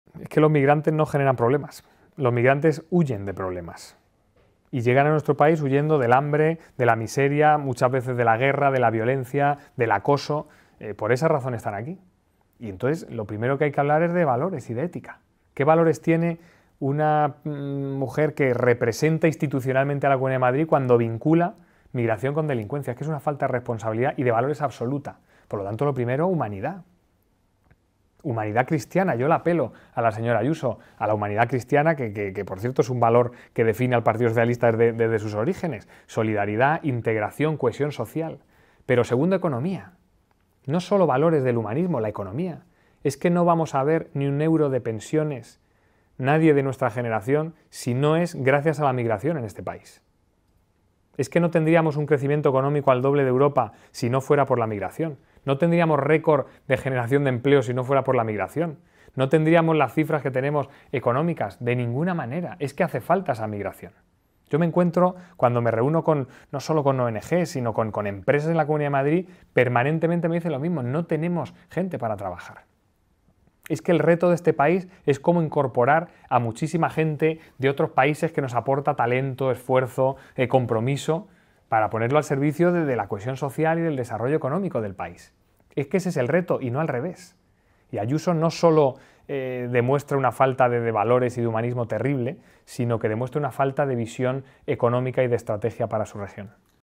Lobato reclama a Ayuso "humanidad cristiana" con las personas migrantes
El secretario general del PSOE-M, Juan Lobato, ha reclamado a la presidenta de la Comunidad de Madrid, Isabel Díaz Ayuso, que tenga "humanidad cristiana" con las personas migrantes, negando que "generen problemas" y recordándole que "huyen de problemas y llegan huyendo del hambre, de la miseria, muchas veces de la guerra, de la violencia y del acoso".